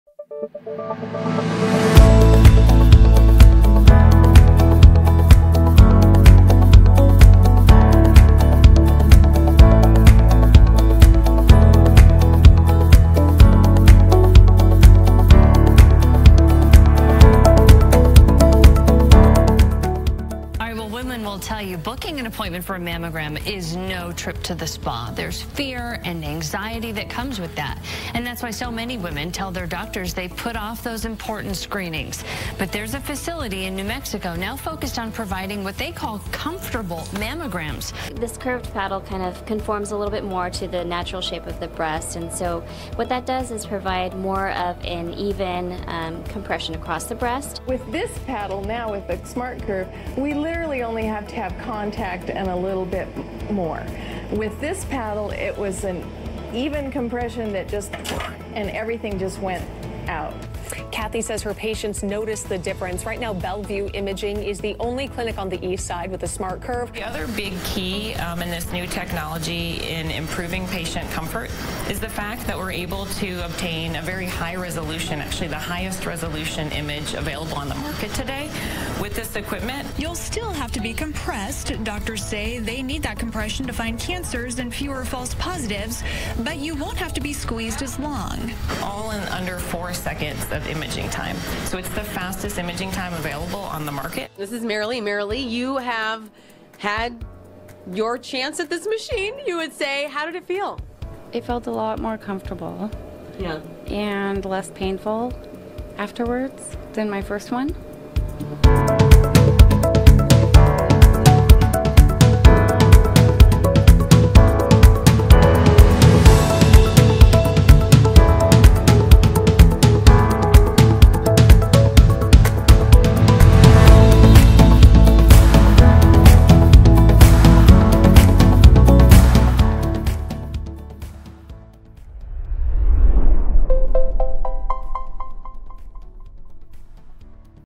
Hologic SmartCurve™ System in the News!
Hologic is committed to educating women about the latest innovations in breast cancer screening, like our SmartCurve™ Breast Stabilization System. That’s why we’re working with customers across the nation to spread the word about a more comfortable mammogram. The buzz is building!